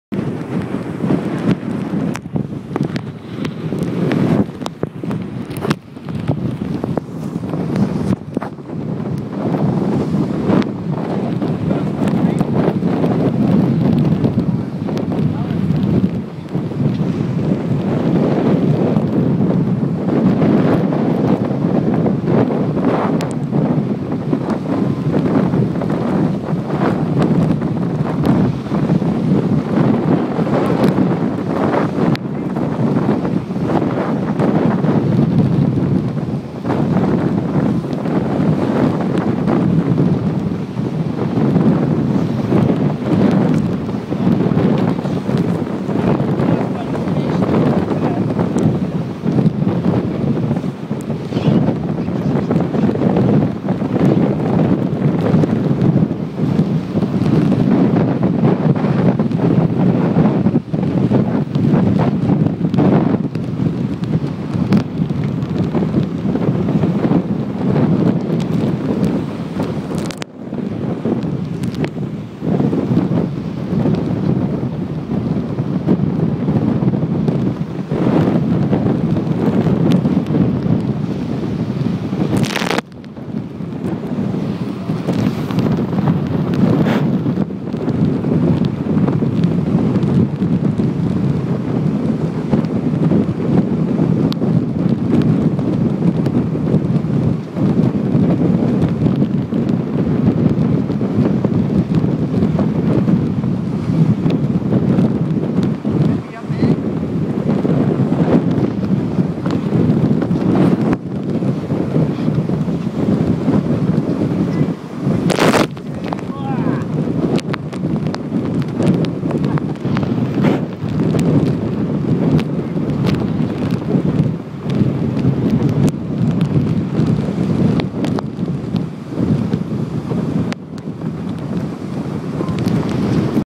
Sea Hawk: Farr 44 IMS - Port Jackson to Port Hacking 6.2.11 (3)
Sea Hawk approaching the finish line after a 35-40 knot southerly change on the return leg from the harbour on 6.2.11